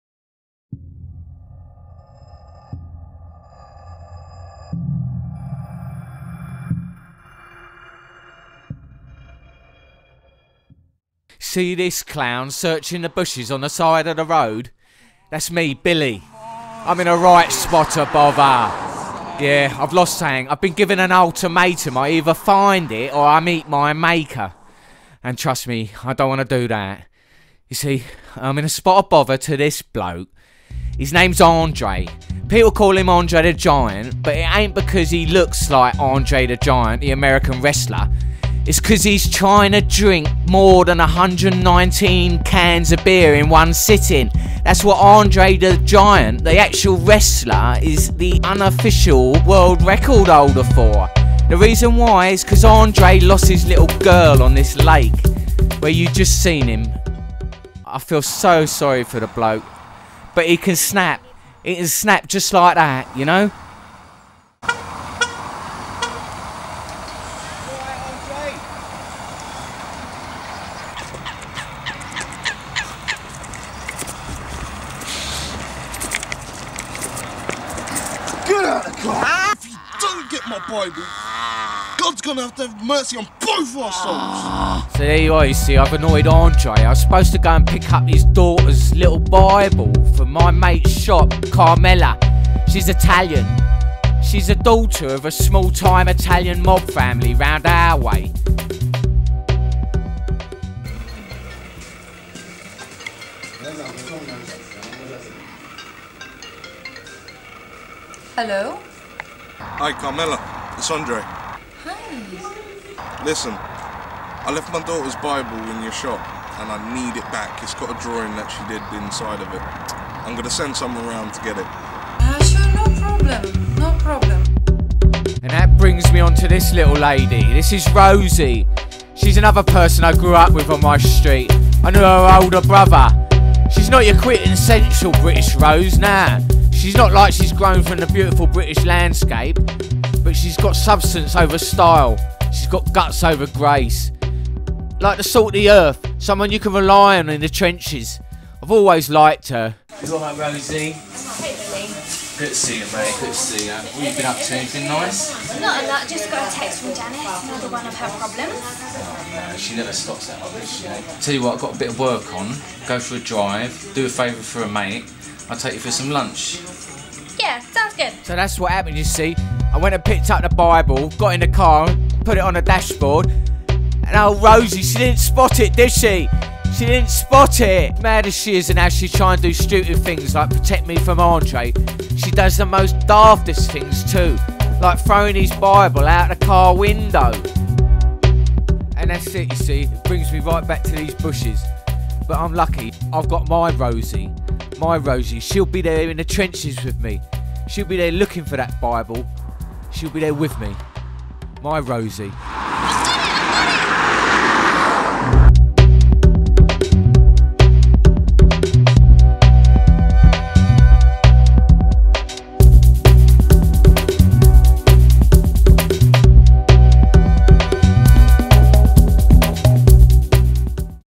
Salt of the earth - (Newfound ground productions 24hr film race entry 2014)
Our entry into the 24 hr film race 2014 at filmracing.com.
Theme given was: An ultimatum
Prop to use was: A broom,
The action to have was : Biting into something.
Limits: Max 3.30mins of film and 30secs credits. 24hrs  to get it all done.